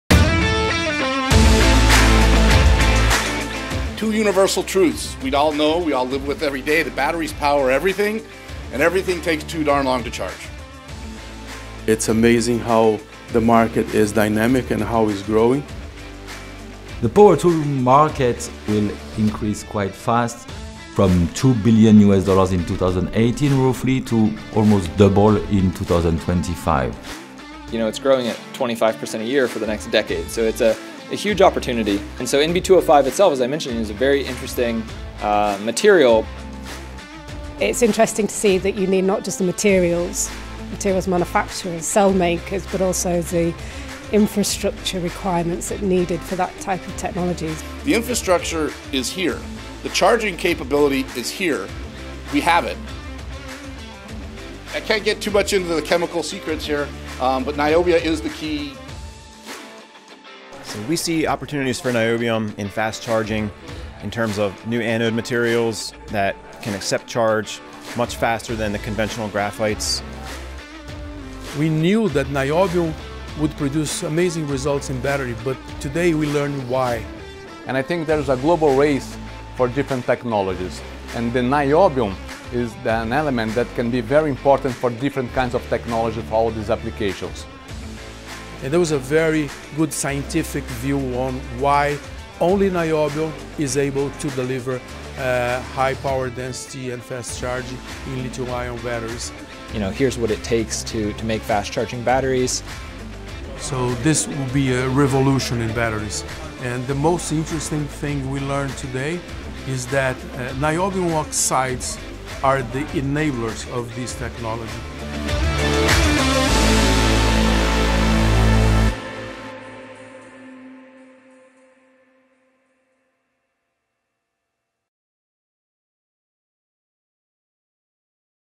Charles Hatchett Highlights 2019
The annual award is selected by a panel of international experts and is ratified by the UK’s Institute of Materials, Minerals and Mining (IOM3). The Award brings together technical experts from around the world and is important to CBMM | Niobium in identifying new applications and markets. To learn more about the Charles Hatchett Award,